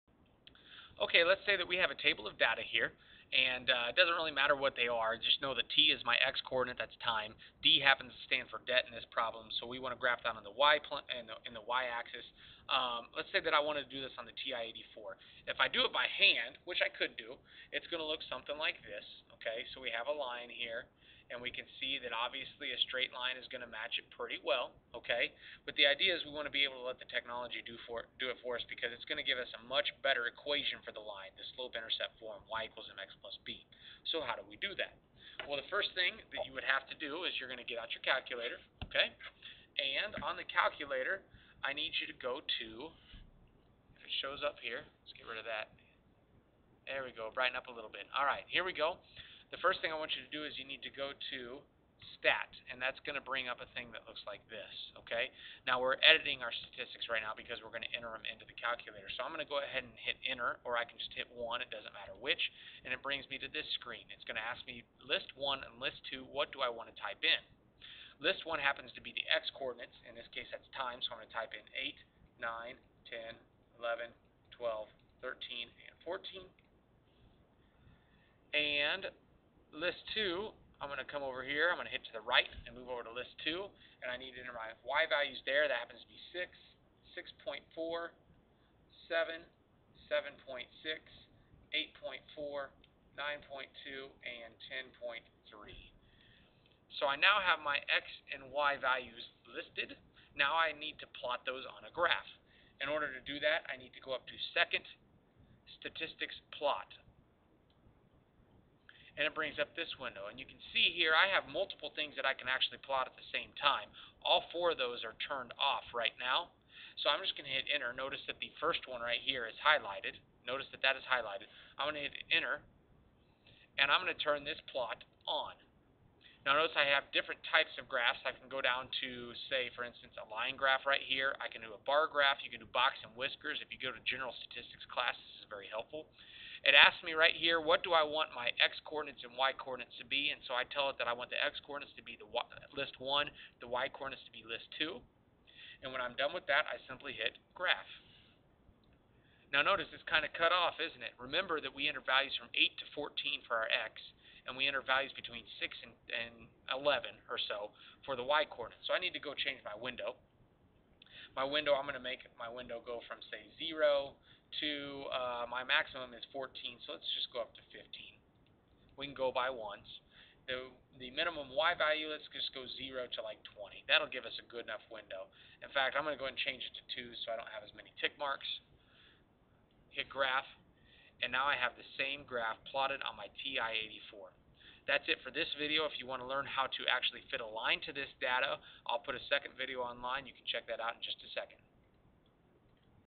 Plotting Data on the TI 84
How to plot statistical data on the TI-84 if the data is given in table form.  This leads into the next video, which deals with finding a best fit line using the Linear Regression features of the TI-84.  Created for my high school Pre-Calculus students.